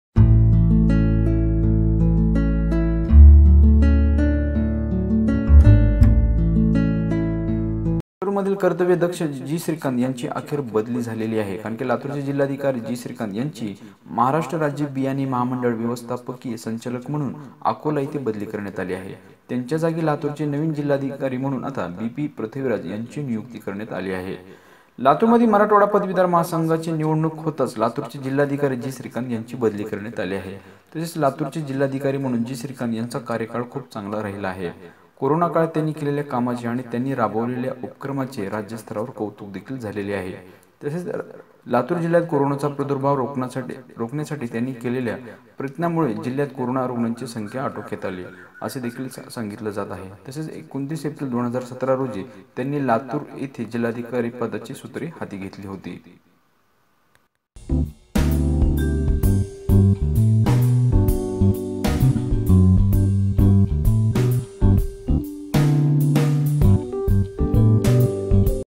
लातूरचे जिल्हाधिकारी जी.श्रीकांत यांची बदली : पहा नवीन जिल्हाधिकारी म्हणून बी.पी. पृथ्वीराज
Latur City New IAS Collector Mr. Prithviraj B. P